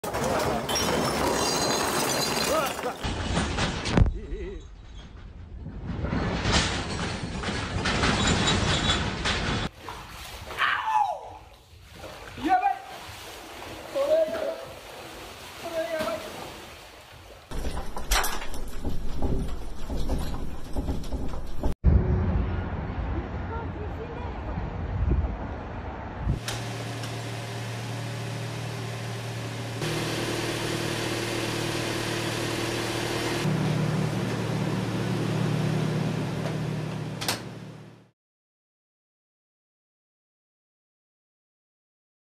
At least 100 injured after M7.3 quake strikes off NE Japan
At least 100 people were injured after a 7.3 magnitude earthquake struck off Japan's northeastern Fukushima Prefecture at 11:08 p.m. local time (1408 GMT) on Saturday, according to the Japan Meteorological Agency (JMA). A tsunami warning has not been issued so far. Tremors were also felt in Tokyo, according to a CGTN reporter in Japan.